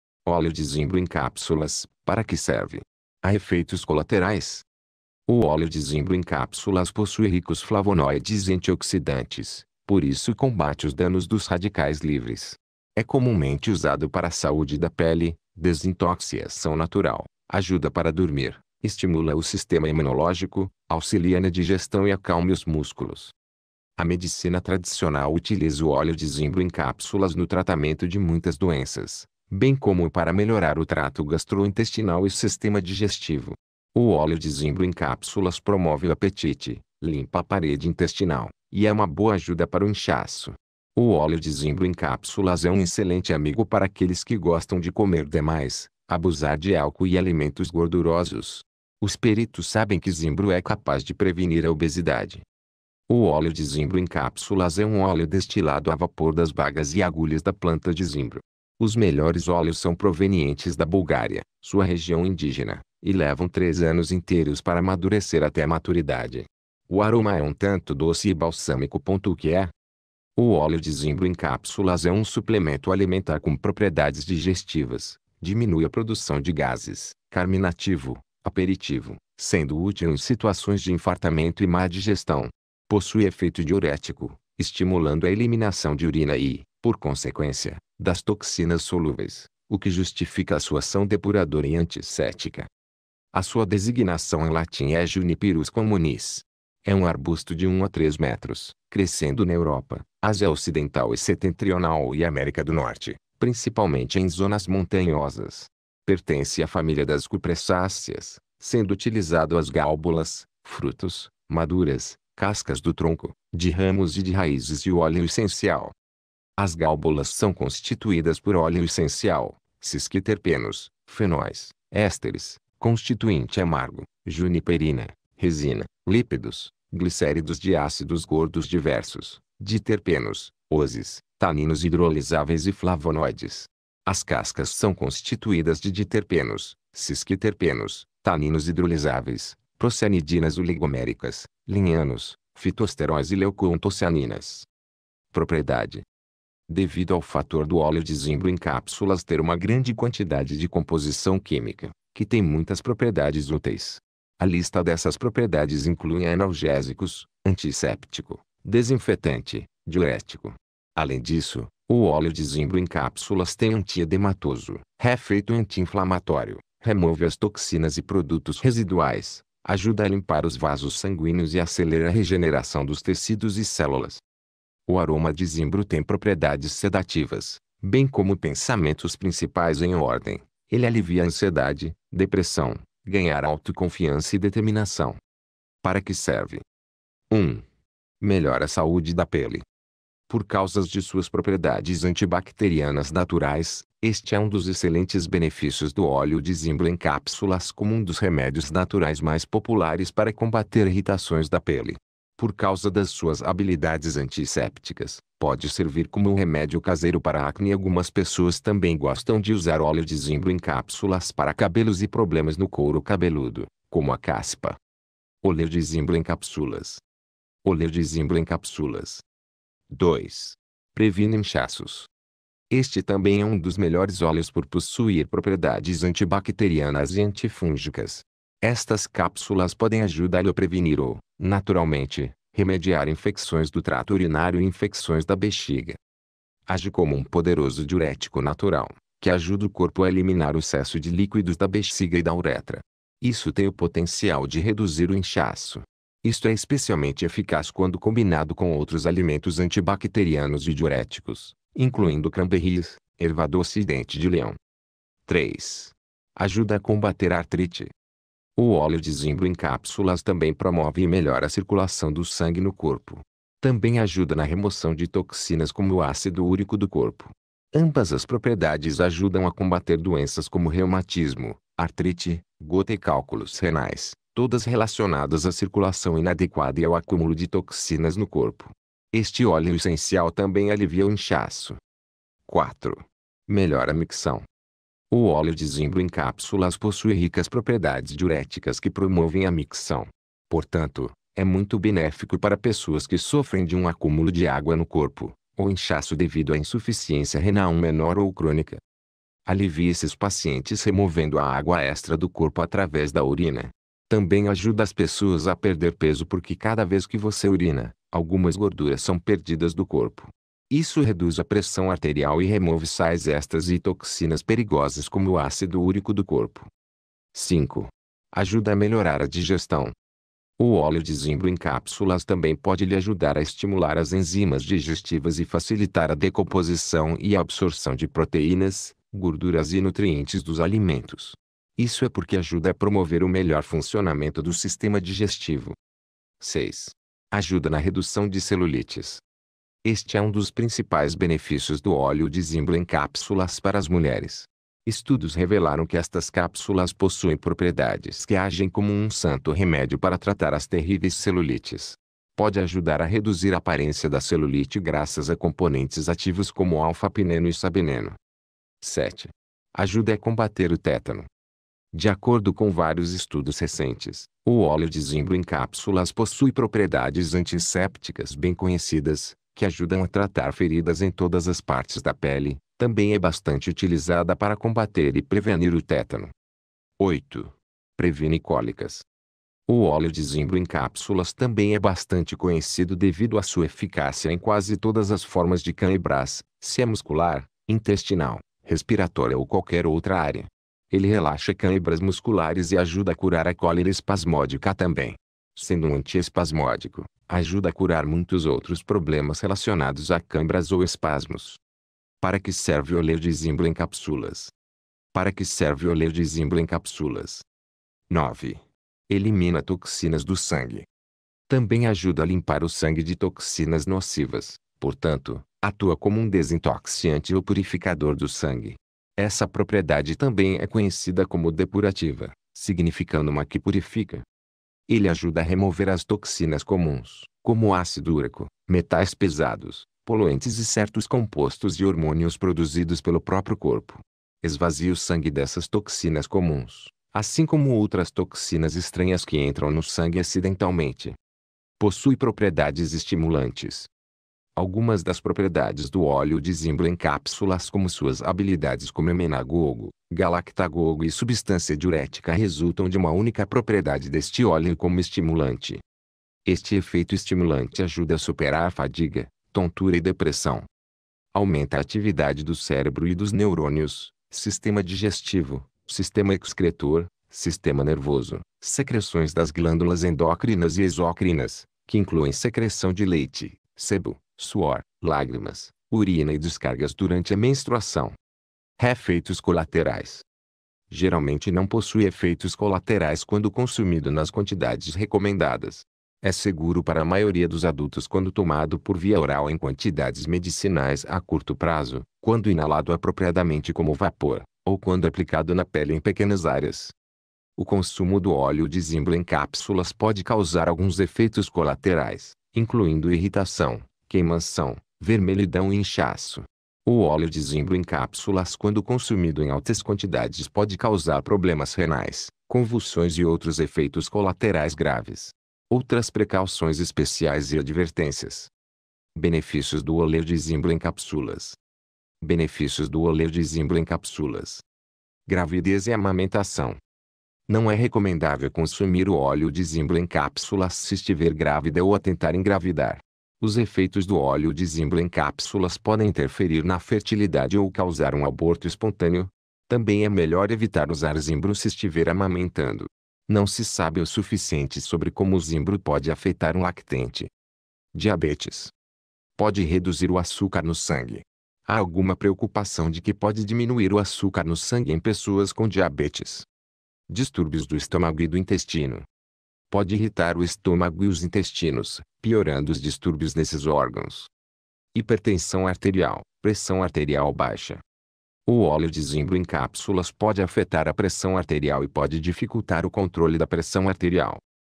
Óleo de zimbro em cápsulas: para que serve? há efeitos colaterais?
O óleo de zimbro em cápsulas possui ricos flavonóides e antioxidantes, por isso combate os danos dos radicais livres. É comumente usado para a saúde da pele, desintoxicação natural, ajuda para dormir, estimula o sistema imunológico, auxilia na digestão e acalma os músculos.